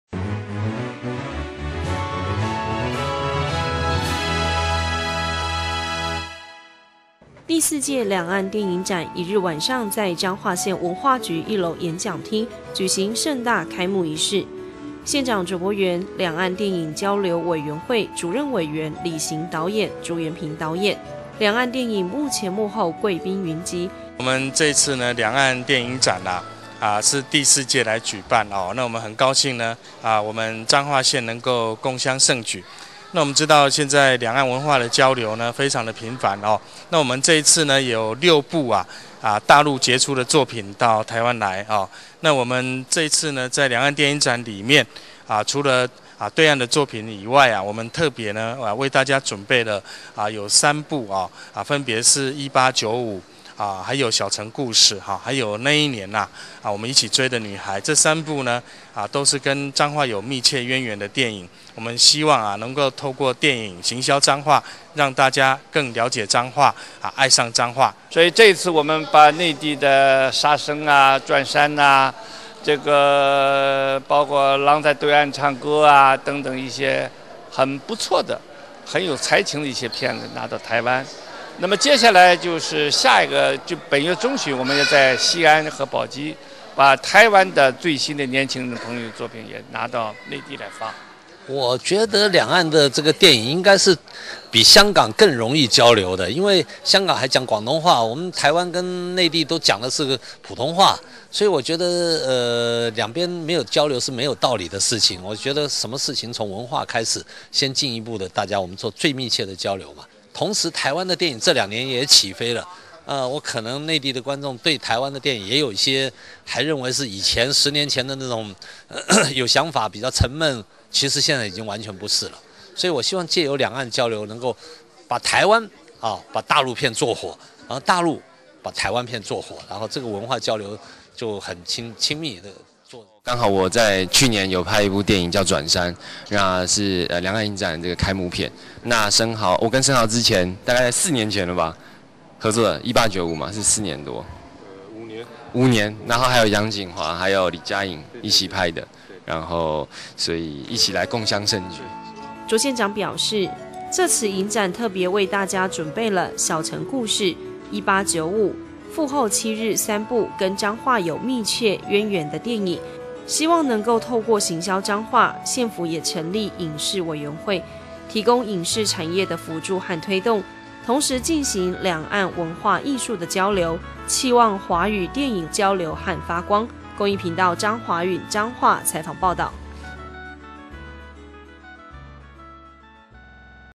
101-06-01 兩岸電影展盛大舉行，開幕片《轉山》吸引滿場觀眾欣賞
第四屆兩岸電影展於1日晚上在彰化縣文化局一樓演講廳舉行盛大開幕儀式。彰化縣長卓伯源、文化部影視及流行音樂產業局副局長禚洪濤、兩岸電影交流委員會主任委員李行導演、朱延平導演、中國電影基會會長李前寬導演、《一八九五》男主角溫昇豪、《轉山》男主角張書豪、《父後七日》女主角王莉雯等貴賓與會，不僅兩岸電影幕前幕後貴賓雲集，現場更吸引了爆滿的觀眾爭睹開幕片《轉山》。